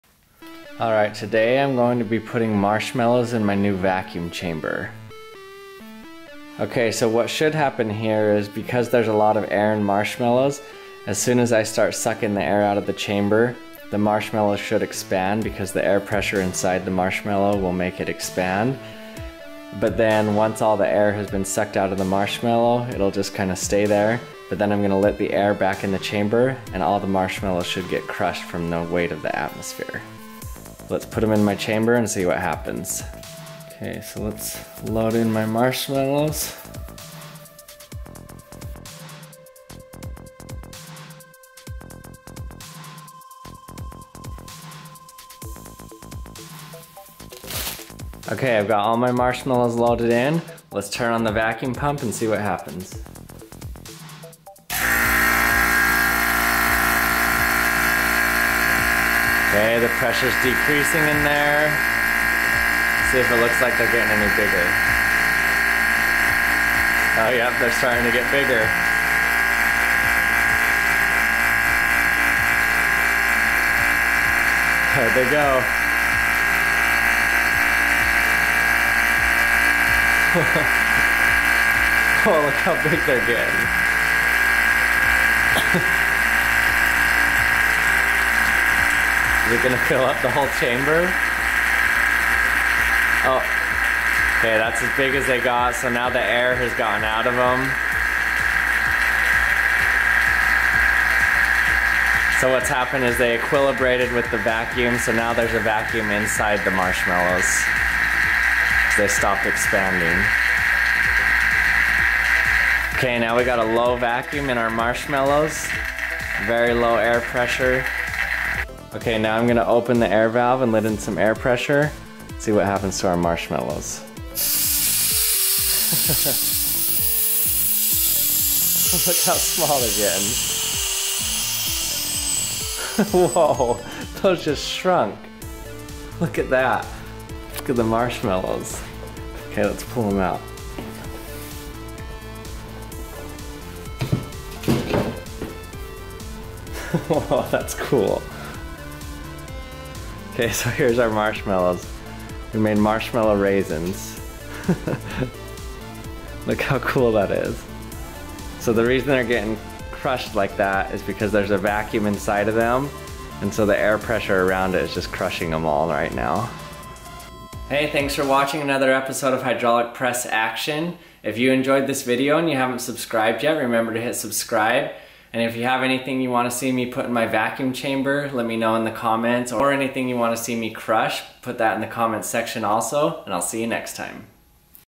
What Happens When You Put 30 Marshmallows In A Huge Vacuum Chamber?
In this video I use my new vacuum chamber to test out what happens when you put marshmallows in a vacuum chamber. I got this vacuum chamber to add to my arsenal of ways to destroy objects and learn about them in the process. So I did not use my hydraulic press to crush the marshmallows, but I they still get crushed in the end. First they puff up due to the air pressure inside of them, then they equilibrate with the vacuum and stop expanding. Then, I open the vacuum and the atmosphere crushed them into marshmallow raisins!